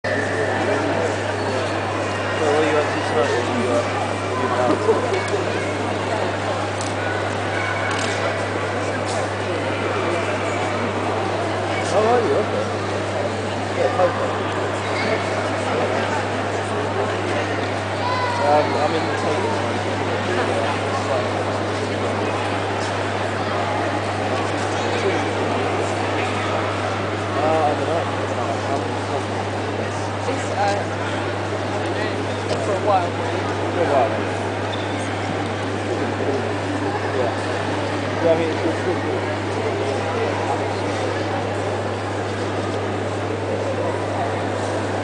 The Weather Project
The Weather Project was a remarkable exhibit at Tate Modern in London. This, hopefully, gives a feel of the scale of it.